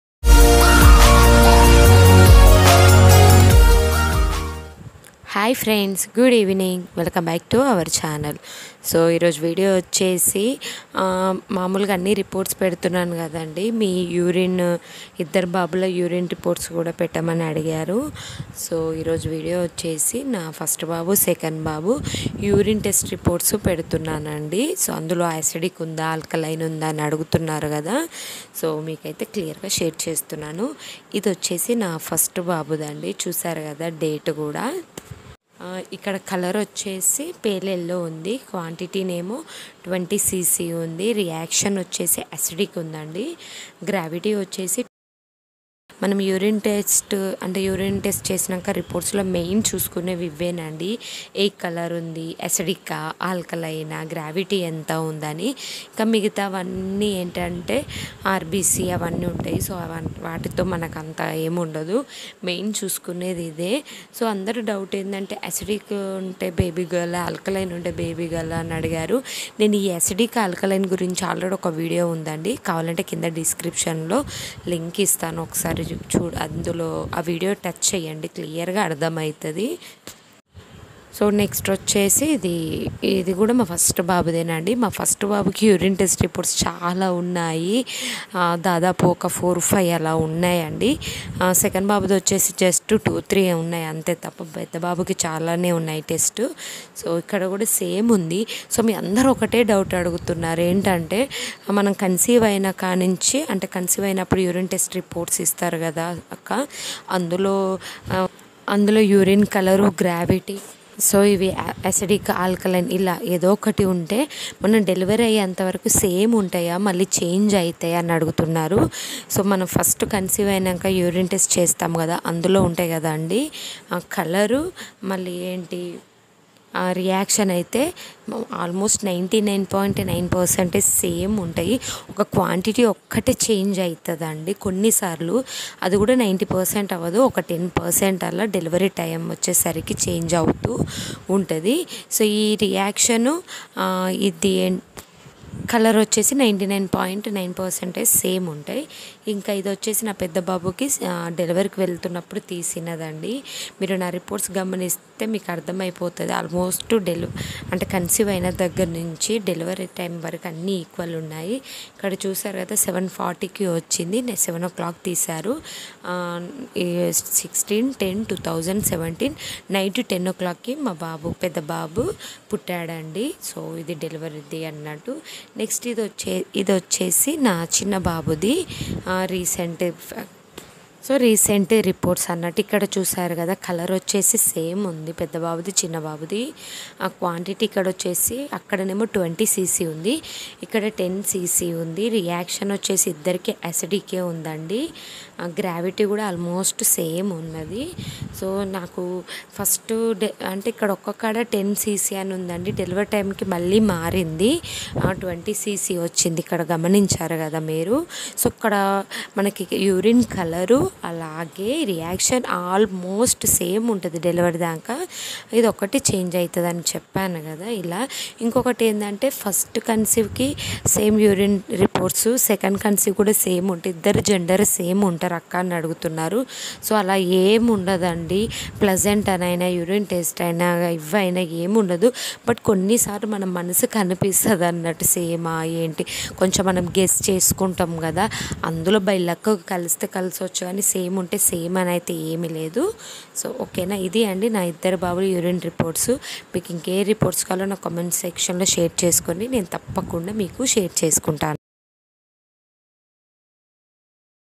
Baby Boys Complete Urine Test Reports//My Baby Boy Urine Reports//Acidic, Alkaline, Gravity, Colour.
Hello Friends 🤗🤗🤗

 YOUTUBE about a Copyright
Infringement..Thank You. "

acidic/alkaline:-